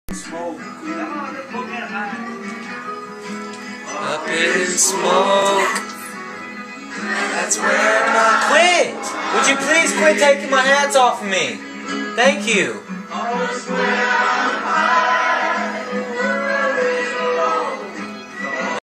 Funny Freakout!!
OK This is my first video in HD!  And my brother wanted me to film him singing.  But his girlfriend was messing with him.  So he explodes in her face!!